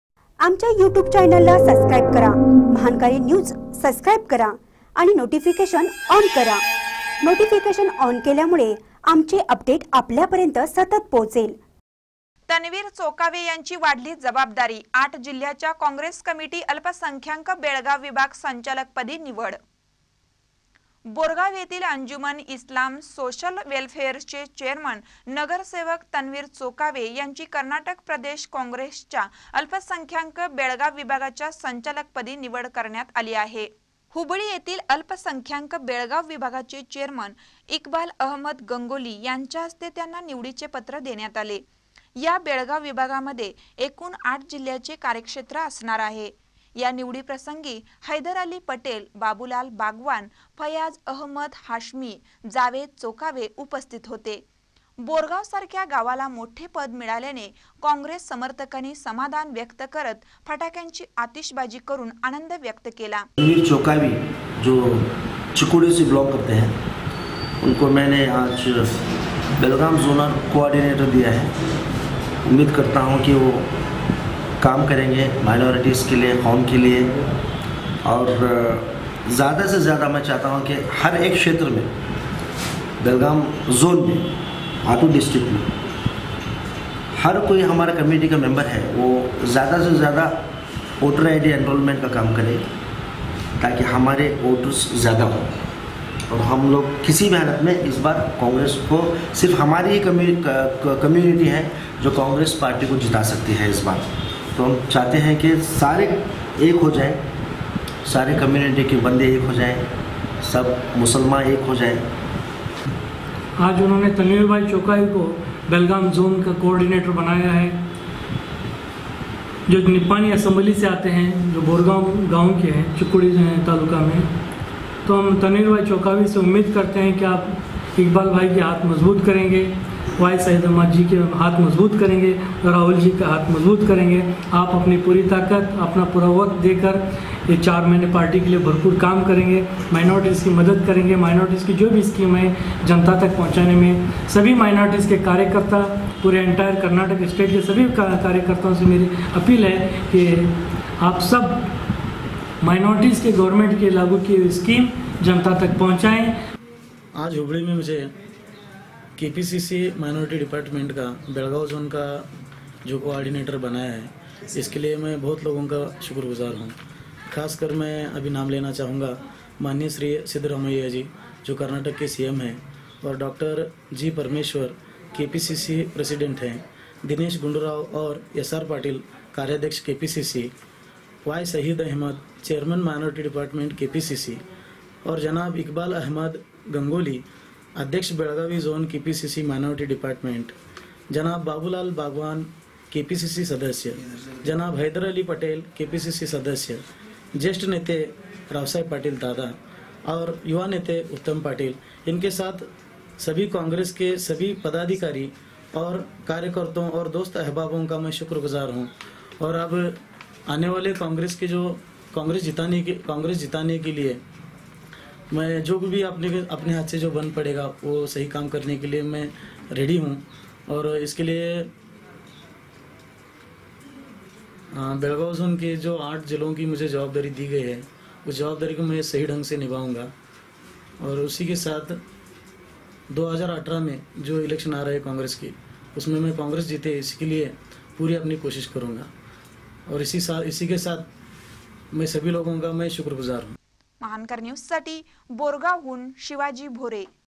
तनवीर चोकावा यांची वाढली जबाबदारी
तनवीर चोकावा यांची वाढली जबाबदारी, आठ जिल्ह्याच्या कॉंग्रेस कमिटी अल्पसंख्याक बेळगांव विभाग संचालकपदी निवड